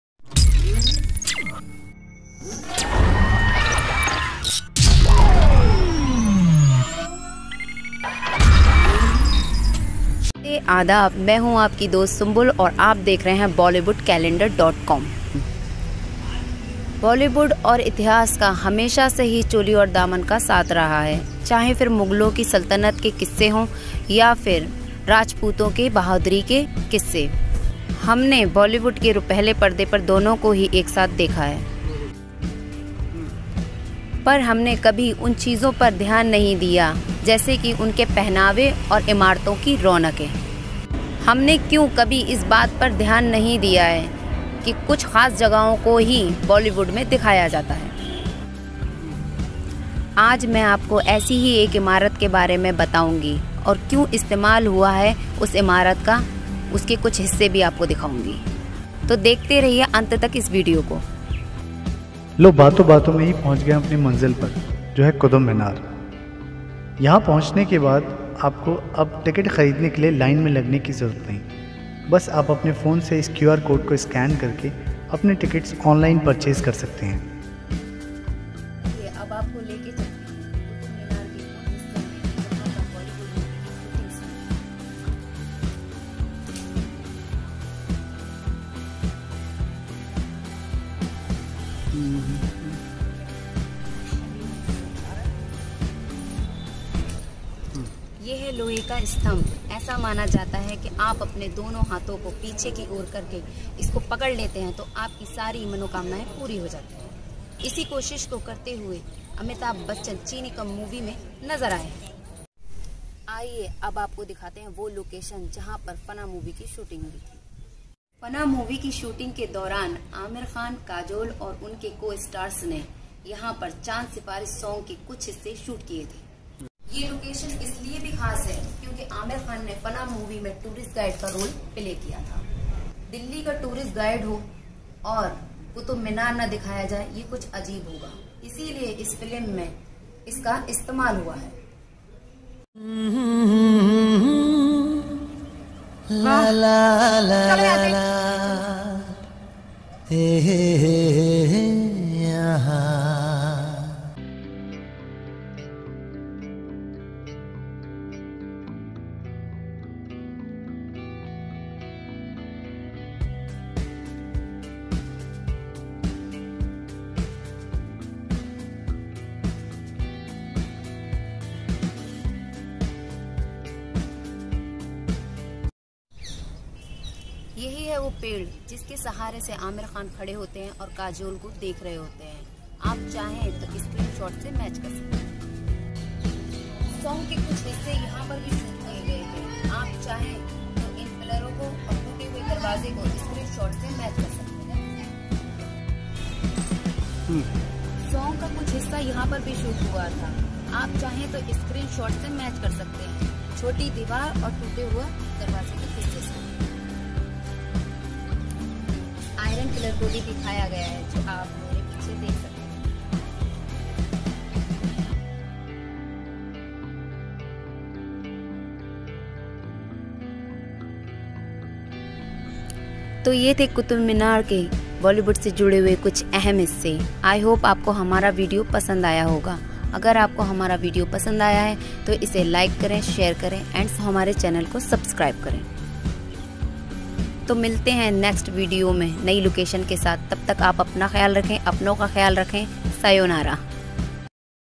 Mysterious Qutub Minar यहाँ पर Shoot हुई थी Movie Fanaa | Seriously Strange I Bollywood Calendar
Bollywood Calendar has shared an amazing shooting location of Bollywood movie Fanaa where Aamir and Kajol have preformed. Qutub Minar is a mysterious place. Qutub Minar has an amazing history as well.
Please follow us on Bollywoodcalendar.com for Bollywood news and entertainment.